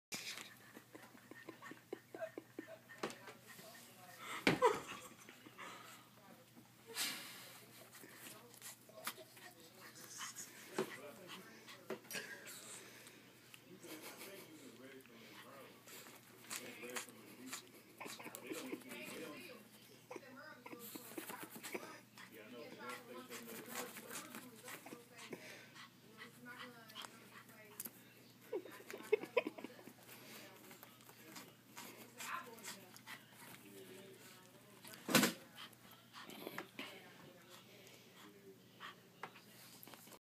"Peanut Meets Bear"
Slim for some reason I just thought this was the funniest thing ever, I walked into the room looking for my mothers dog named Peanut & although I couldn't reach for my phone fast enough this is the remainder of what I found :-/ Gotta be America's Funniest Video or WorldStar material the funniest part is near the end when he noticed the bear wasn't real